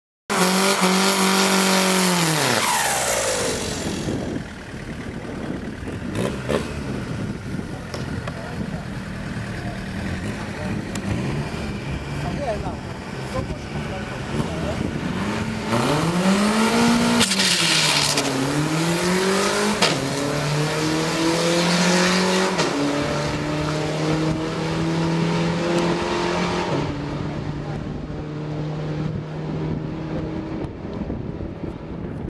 September 2016 QTM Day 1 Run 29
16th September 2016 - Day 1 "Test and Tune" of the massive three day event AKA The Top Aces Event. Lots of new cars to watch, so sit back and enjoy :D